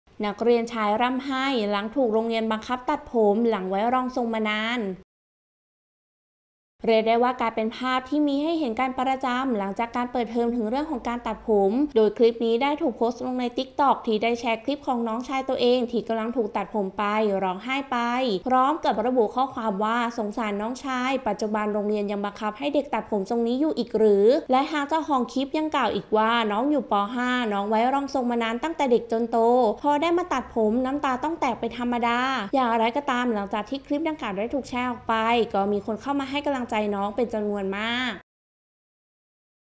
นักเรียนชายร้องไห้หลังถูกโรงเรียนบังคับตัดผม หลังไว้รองทรงมานาน
เรียกได้ว่ากลายเป็นภาพที่มีให้เห็นกันประจำหลังจากการเปิดเทอมถึงเรื่องของการตัดผม โดยคลิปนี้ถูกโพสต์ลงใน Tiktok ที่ได้แชร์คลิปของน้องชายตัวเองที่กำลังถูกตัดผมไปร้องไห้ไป